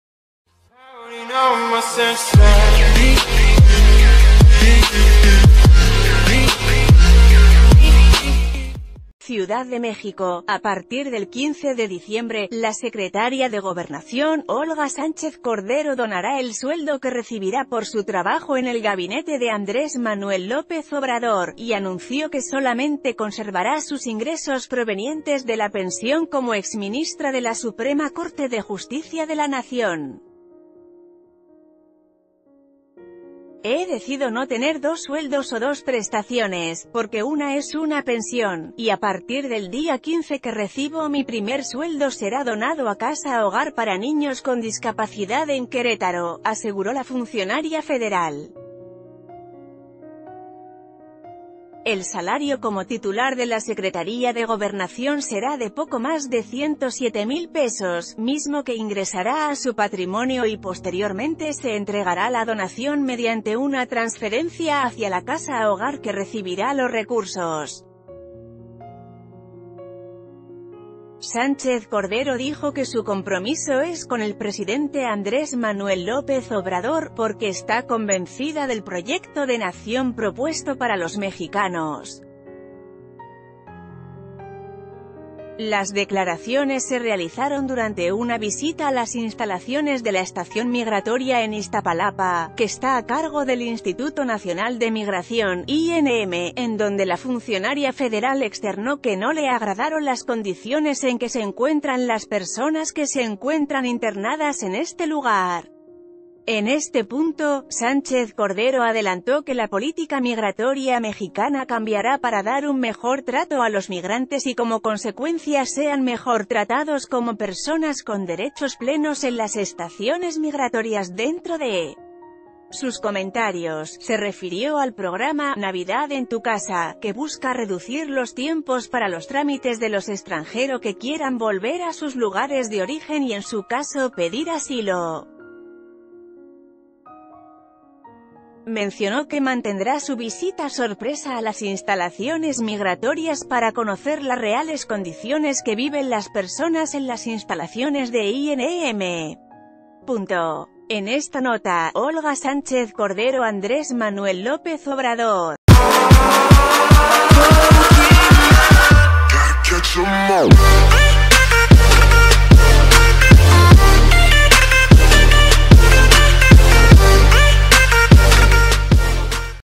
Olga Sánchez Cordero renuncia a su salario en el Gabinete de AMLO
Ciudad de México.- A partir del 15 de diciembre, la Secretaria de Gobernación, Olga Sánchez Cordero donará el sueldo que recibirá por su trabajo en el Gabinete de Andrés Manuel López Obrador, y anunció que solamente conservará sus ingresos provenientes de la pensión como exministra de la Suprema Cor...